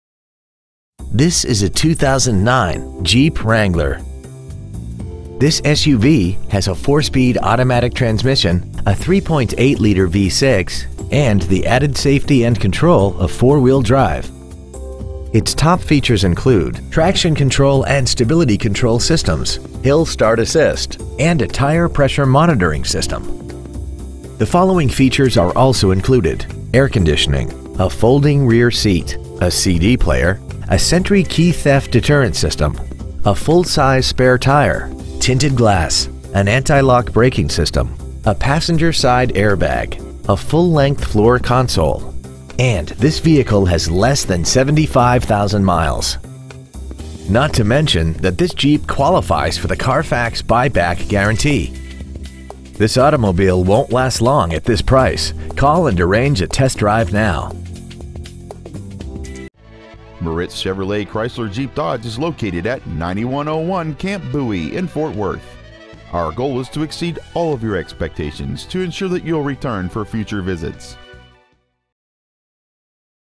2009 Jeep Wrangler Dallas TX
Year : 2009
 Make : Jeep
 Model : Wrangler
 Engine : Gas V6 3.8L/231
 Trans . : Automatic
 Exterior : Detonator Yellow
 Miles : 74,722

 Pre-Owned 2009 Jeep Wrangler Ft Worth TX